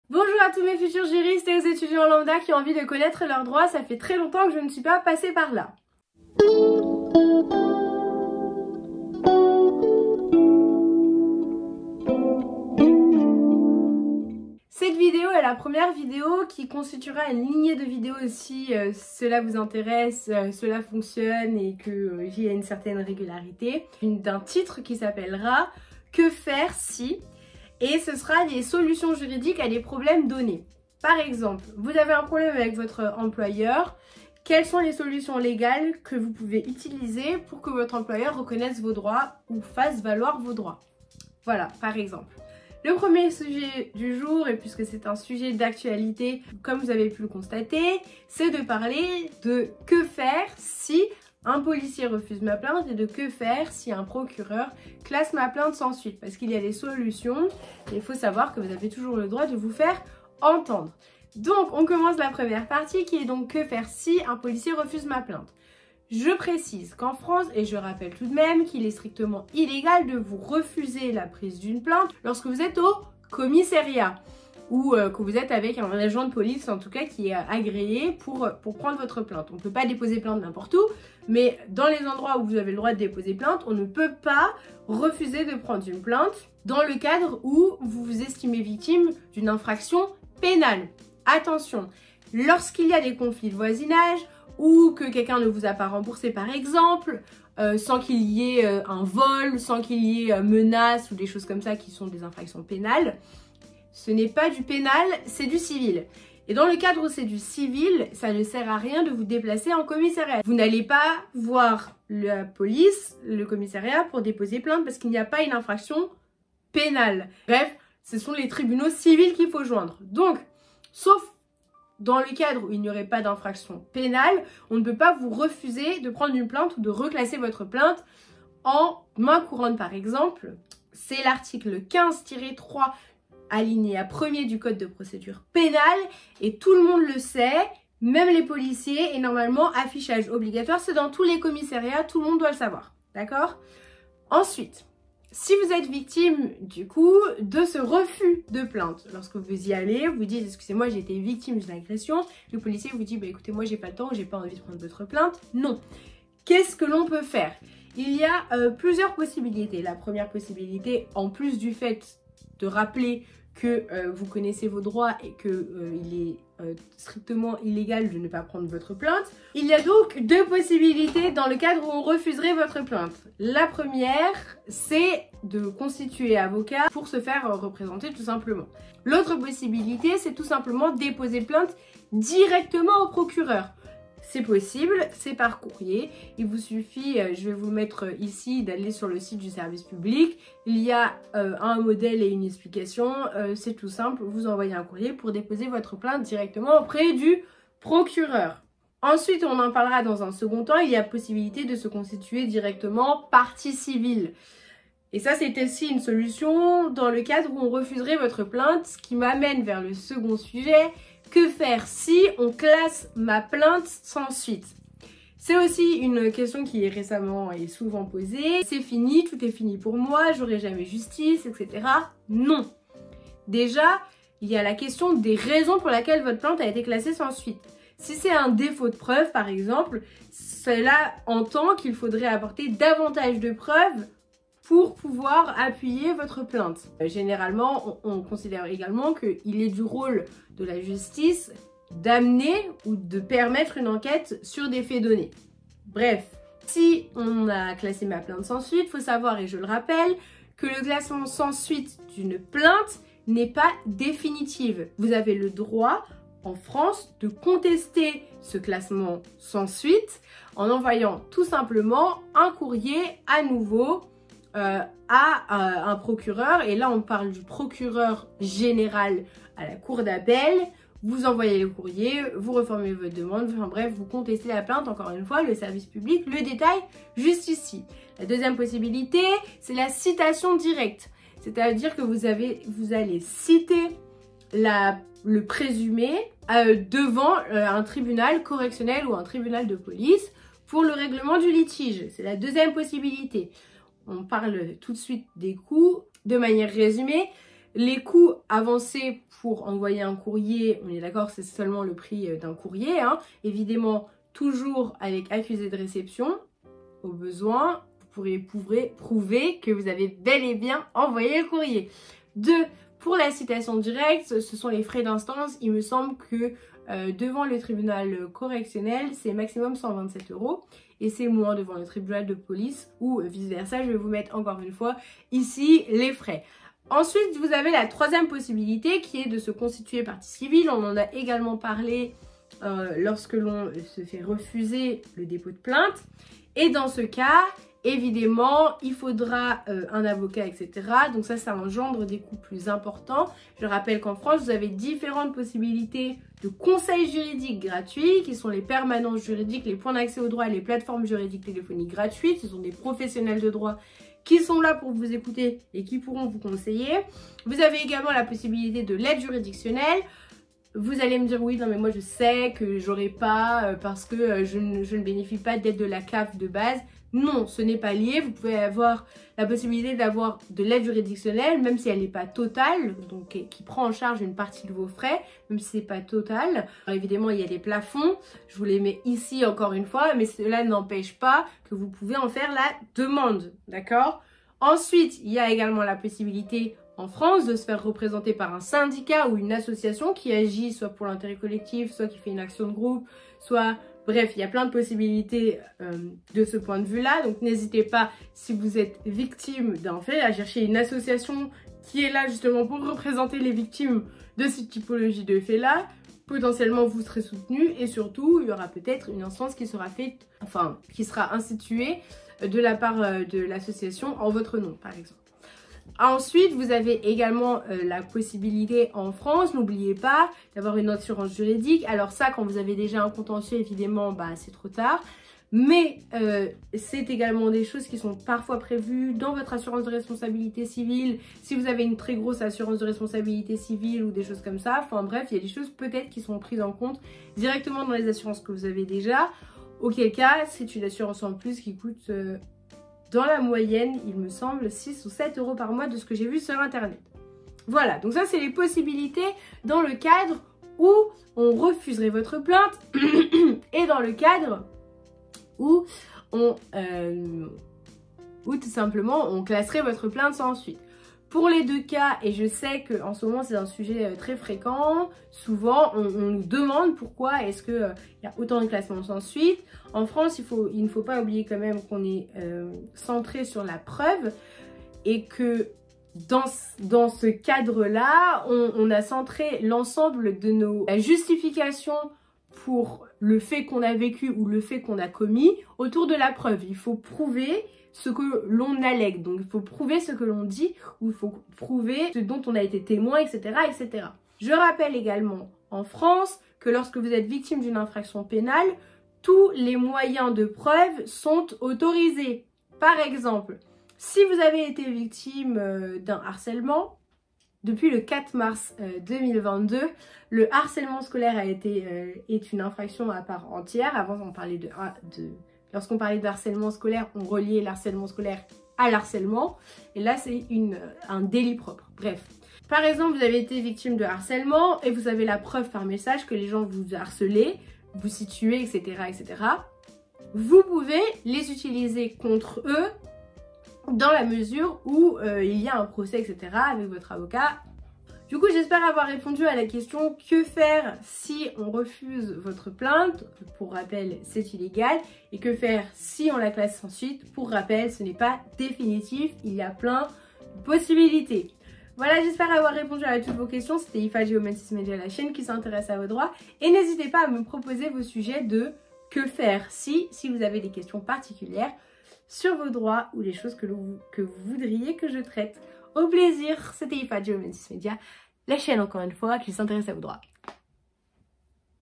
Que faire si...Refus de ma plainte ? Classement sans suite ?
Bonjour à mes futurs juristes et étudiants lambdas qui veulent connaitre leurs droits.  😊 Déroule pour la suite. En vous remerciant pour votre enthousiasme et votre attention 😊
*Attention les informations que je donne ne sont pas exhaustives et sont tirées de ce que je lis si elles ne sont pas complètes ou mauvaises pensez à me le signaler! *

Géomancis Média by ICG tient ses sources du service public.fr; légifrance, tribunaldeparis.fr; gouvernement.fr; Ooreka.
Le But de cette vidéo est de vous parler de toute vos possibilités si l'on refuse ou classe sans suite votre plainte.

Les personnes présentes activement sur cette vidéo sont :
Ifa CG – 22ans – Etudiante en Droit  (3e)
Créatrice de la chaine, monteuse et réalisatrice de la vidéo.

Date de sortie de la prochaine vidéo :
Dimanche 27 mars 2022
Rendez-vous tous les Mercredis et Dimanches.